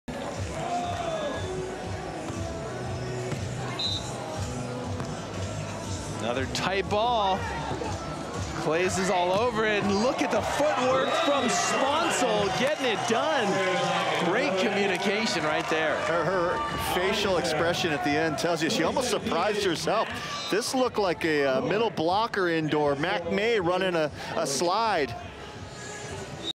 Great Defense Turns Into Great Offense For Claes/Sponcil | 2021 Gold Series Manhattan Beach Open
Kelly Claes’ stellar defense sets the tone for Sarah Sponcil’s powerful kill against Lexy Denaburg and Maria Salgado at the 2021 Gold Series Manhattan Beach Open.